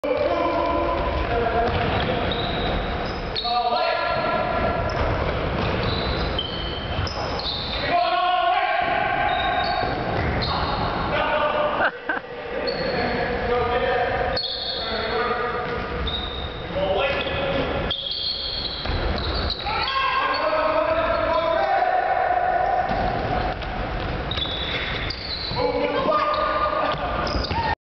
STFX KinBall Society March 2010
KinBall society of St.Francis Xavier University playing a two team version of the game.